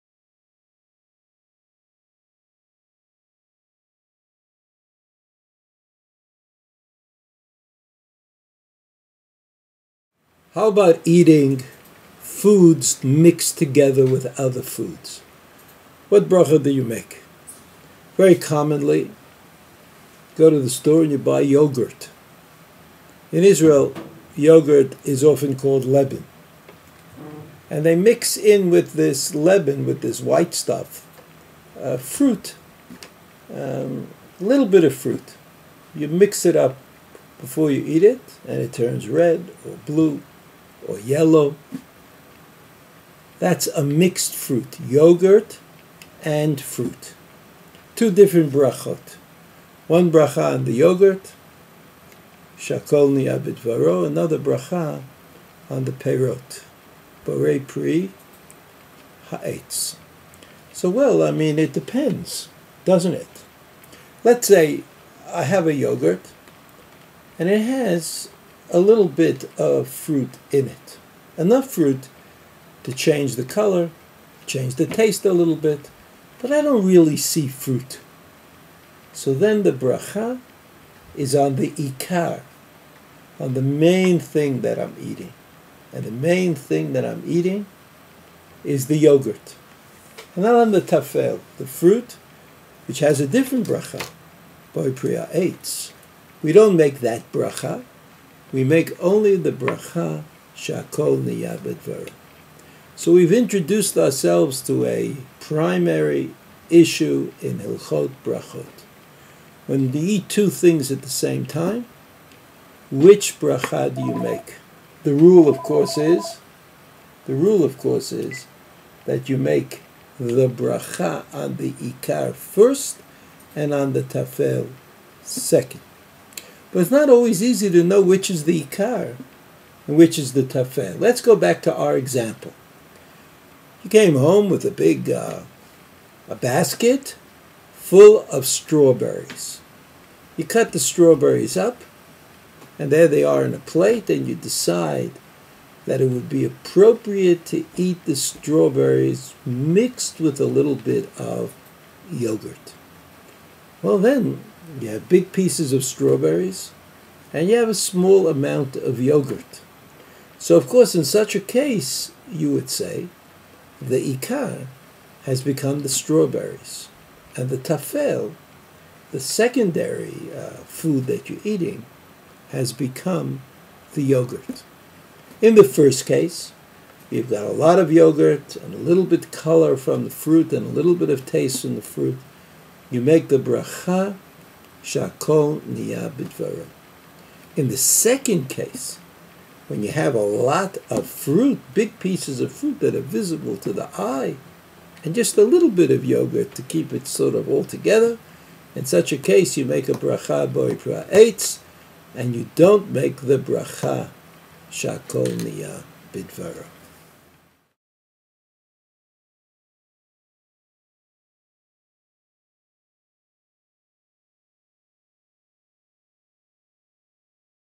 Jewish Food Blessings on Items Mixed Together
To learn more about WebYeshiva or access our full catalog of free and interactive online classes please
In this video: Rabbi Chaim Brovender, of WebYeshiva.org discusses what Jewish food blessings to make on a food that is mixed with other foods. Rabbi Brovender introduces the concept of ikar and tafel.